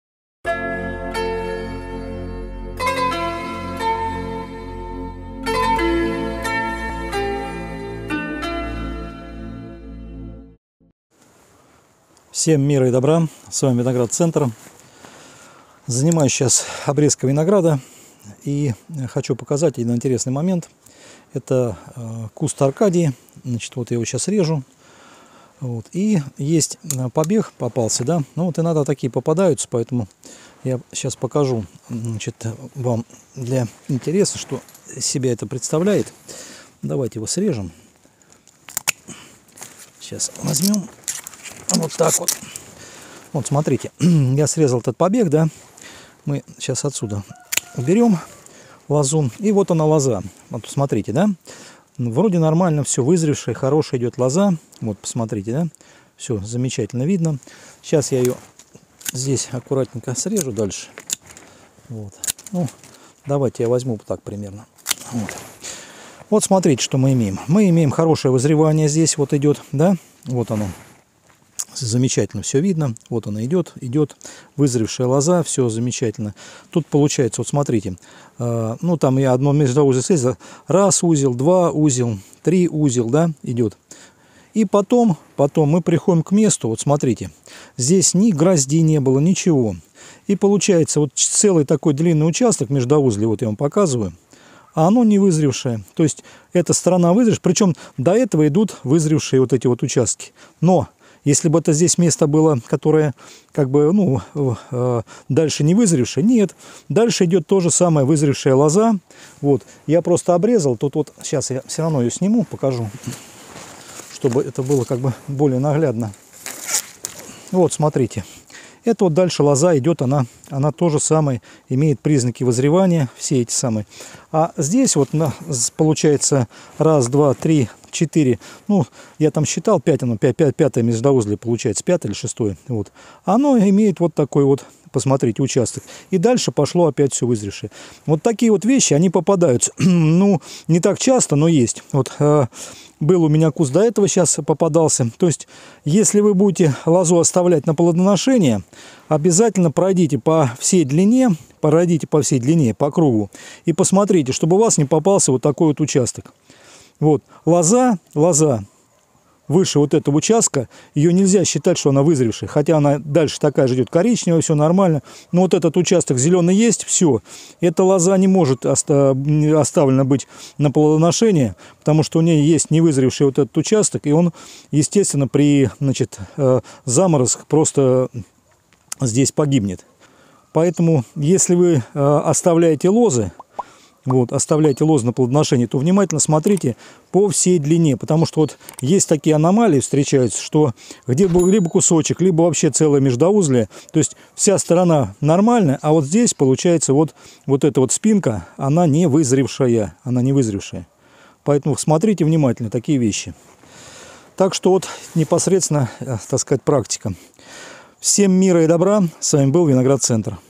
Повод веский- лоза, которую нельзя оставлять при обрезке A vine that should not be left when pruning
Из настоящего видео Вы узнаете о том, какую лозу нельзя оставлять на плодоношениие следующего года.
Ссылка на текстовый вариант видео "Обрезка молодого куста винограда для "чайников"